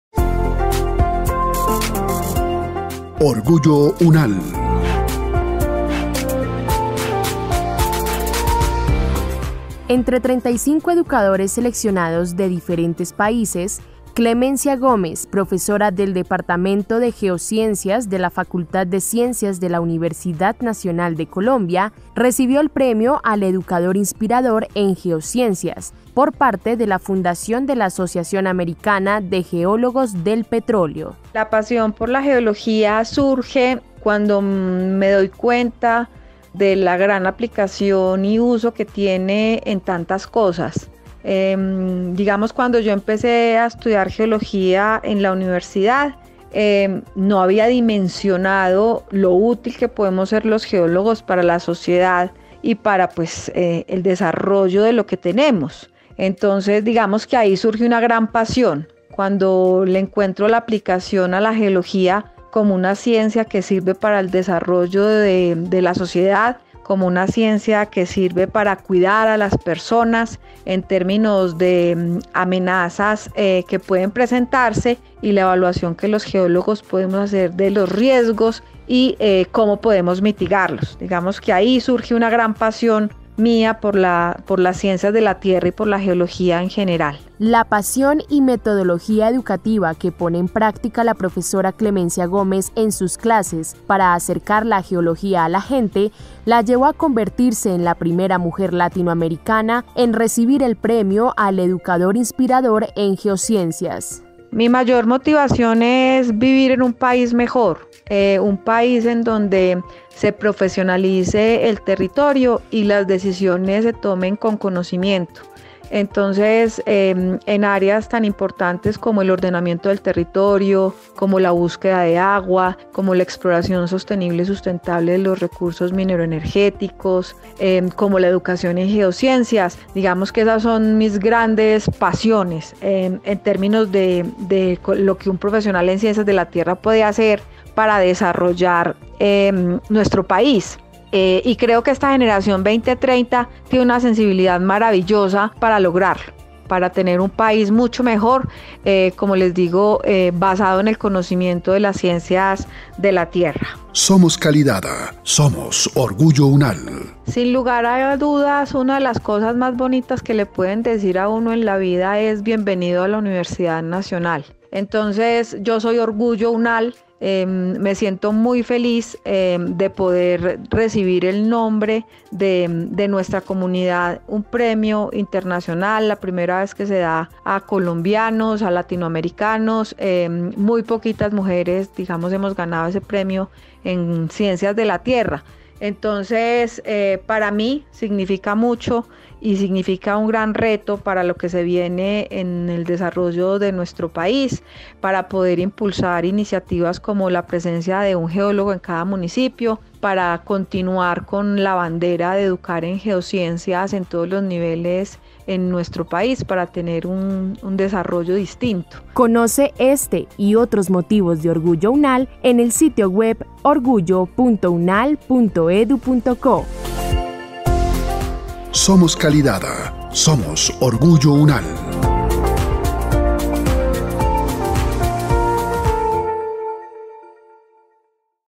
OrgulloUNAL | Premio para profesora de la UNAL
SomosOrgulloUNAL La profesora Clemencia Gómez, recibe premio americano por inspirar a sus alumnos